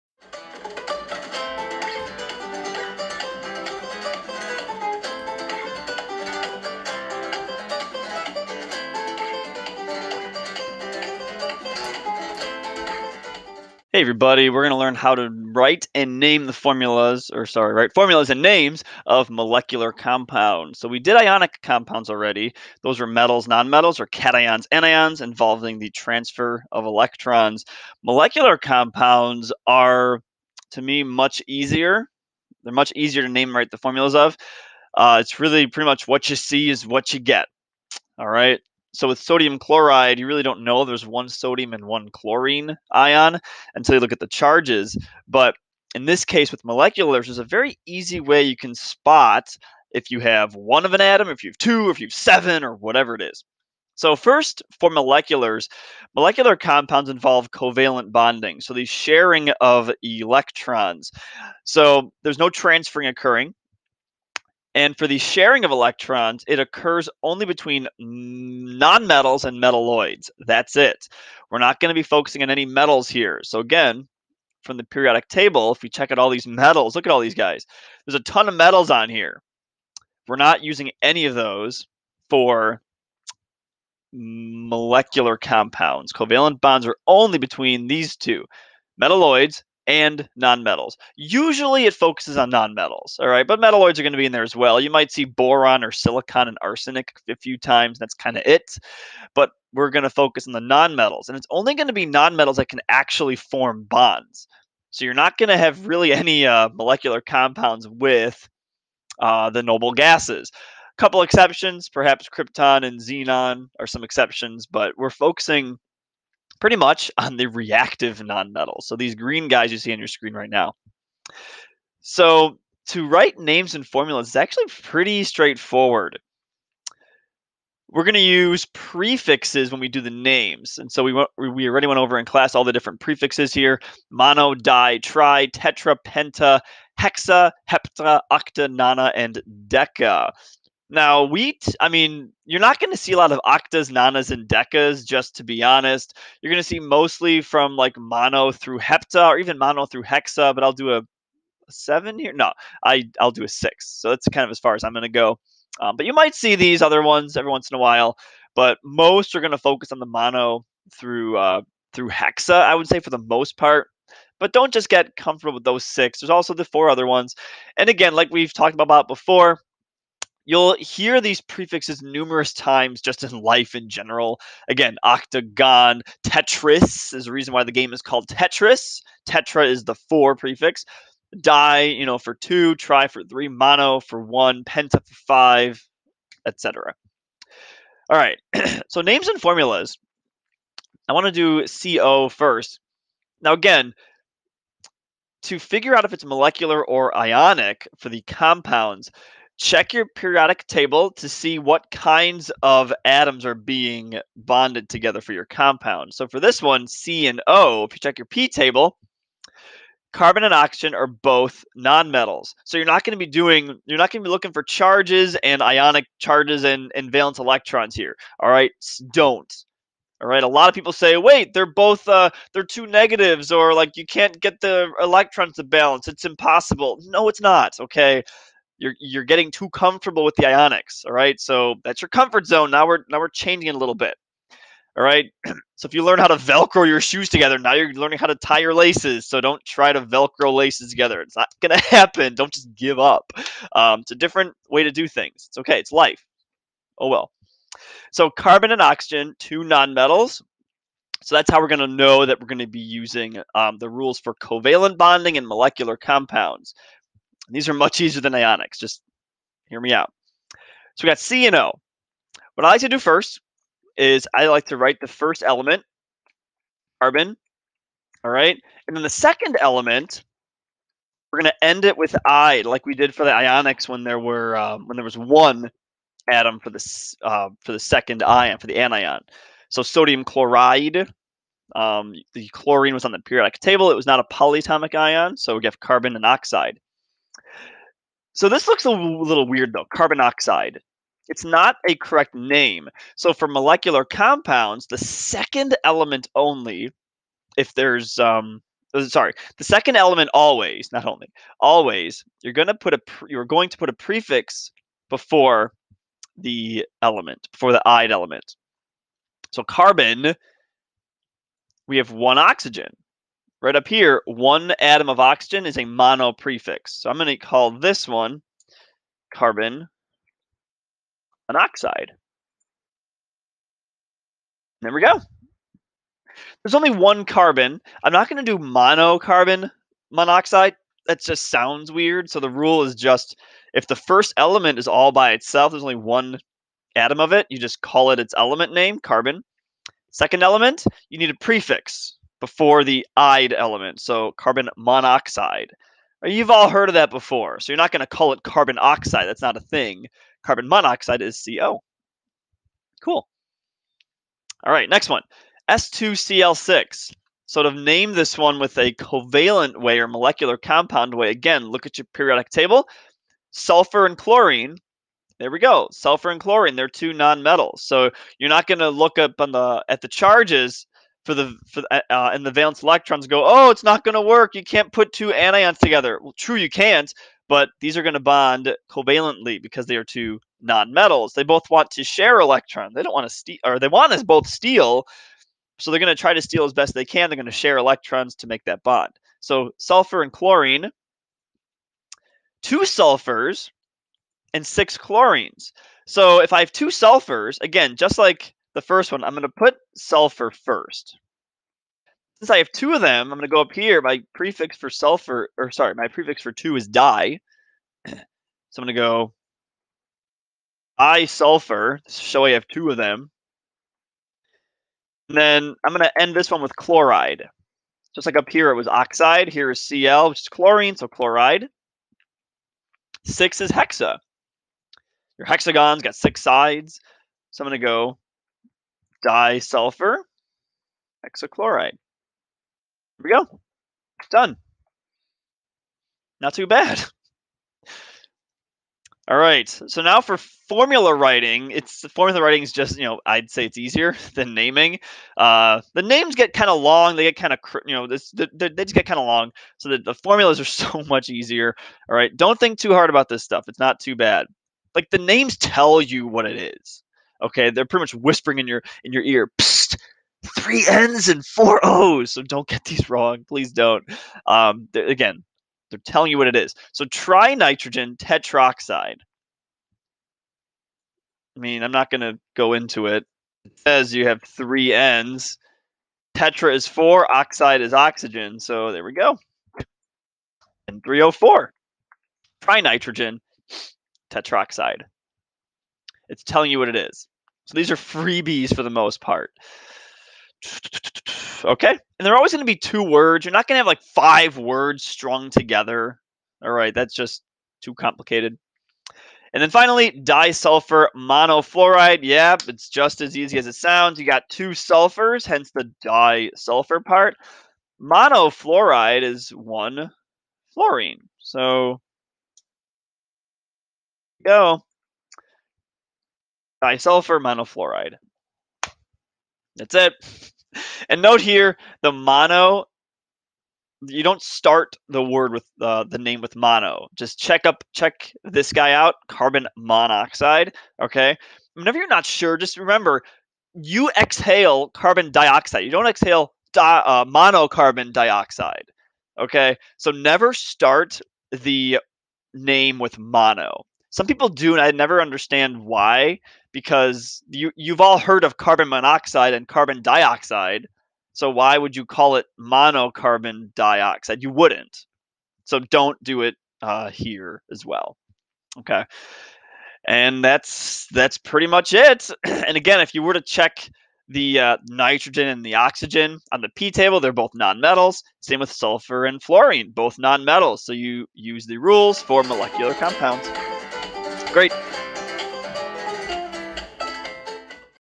Molecular Compounds (Names & Formulas)
Check out out to write & name properly for MOLECULAR COMPOUNDS!  It's not too shabby at all!